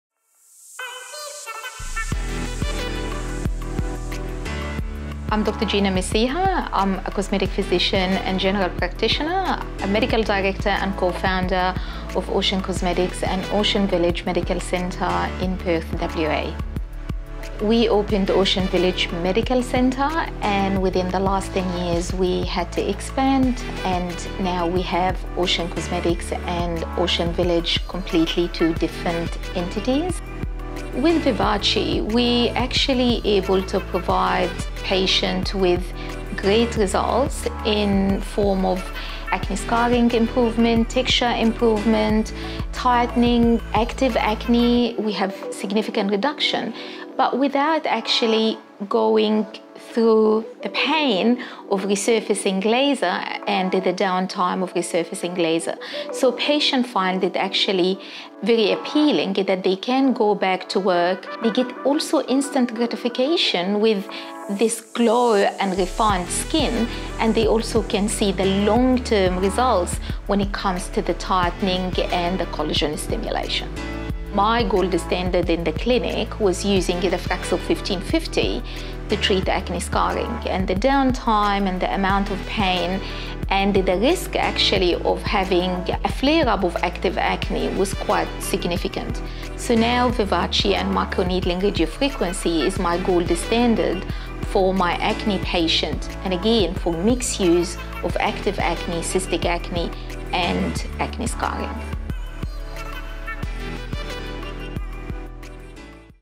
Ocean Cosmetics - Vivace
Dr Gina Messiha discusses the versatility of Vivace and why it’s a key technology in her clinic. With Vivace, you can effectively treat a variety of common concerns such as wrinkles, active acne, scarring, poor skin texture, skin tightening and more!